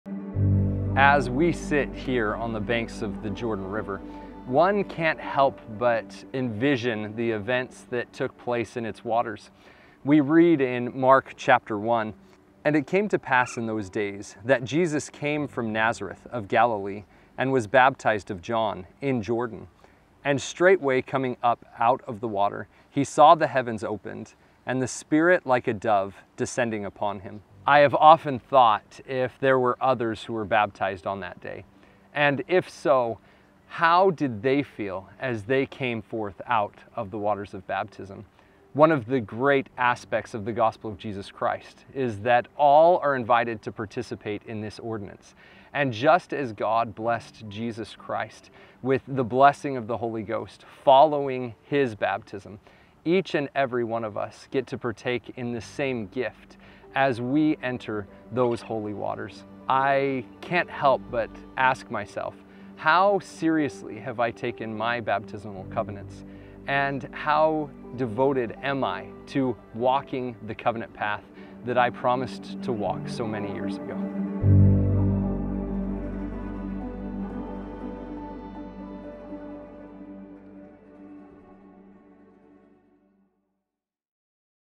The Baptism of Jesus
Josh Matson reflects on the covenant of baptism, and Christ's example, from the banks of the Jordan River.